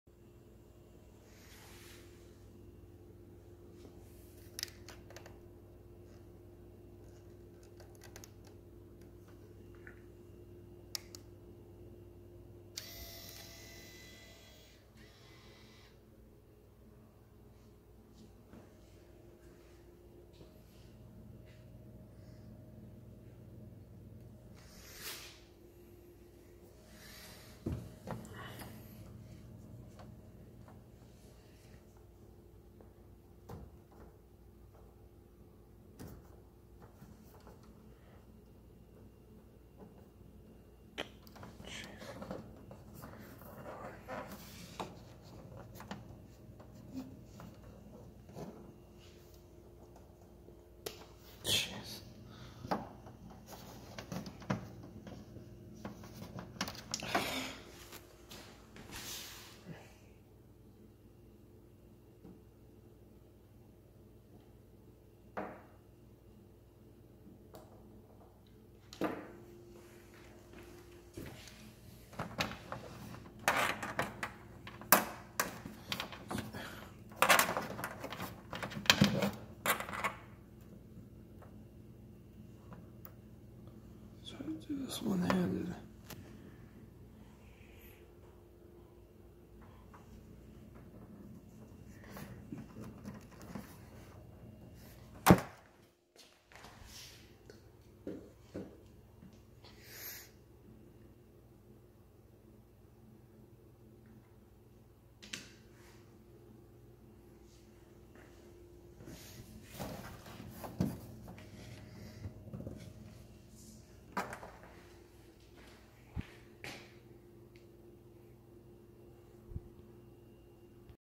Wii no signal Samsung TV FIX!!!
I was just messing with the av adapter and I discovered that switching the white and yellow plugs will do the trick.